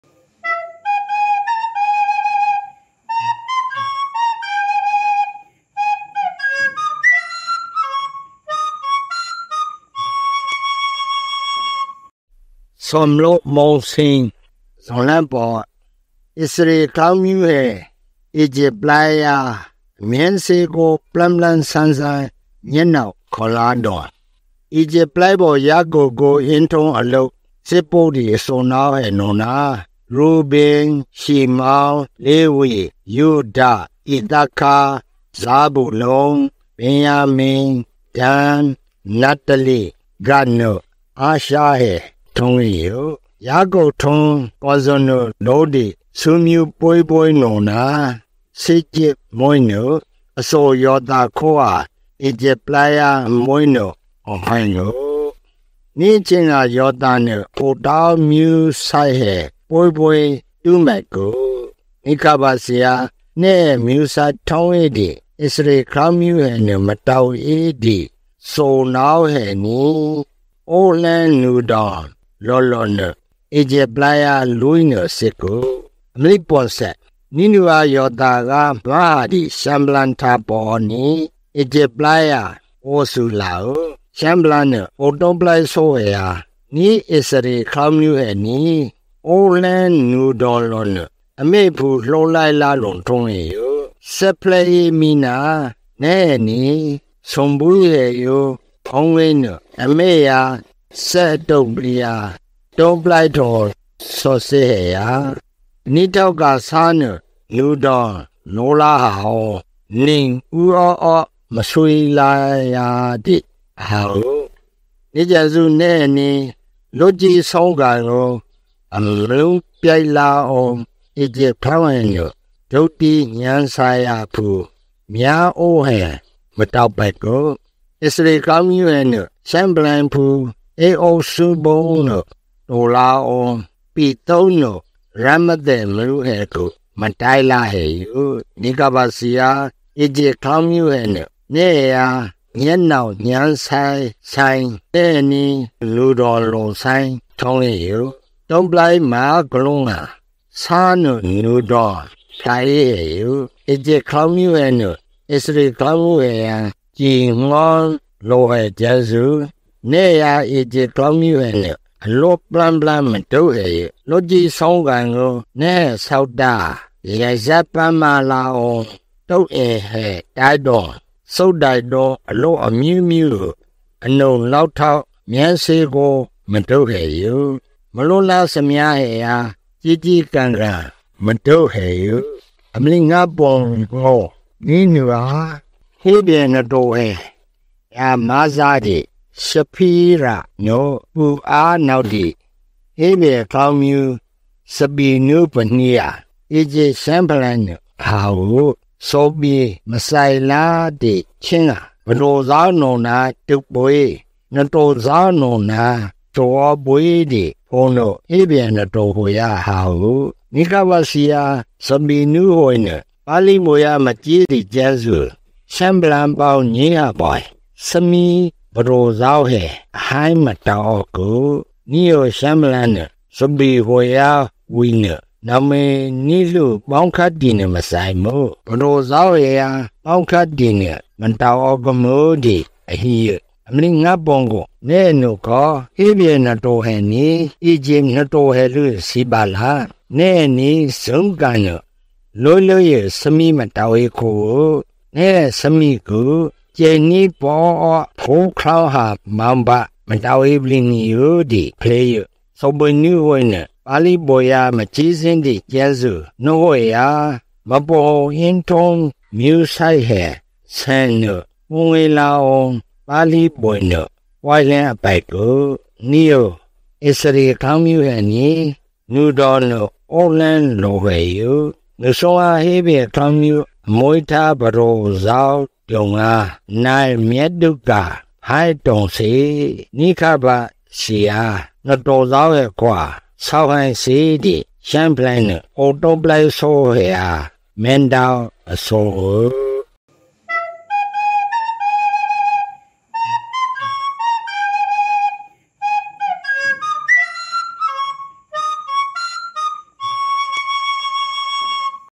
Exodus 01, read by Salai Kyaw Sein
အရှိုကျမ်းစာ ထွက်မြောက်ရာကျမ်းစာကို ဦး' ဆလိုင်း ကျော်စိန် က ဖတ်ကြားပါတယ်။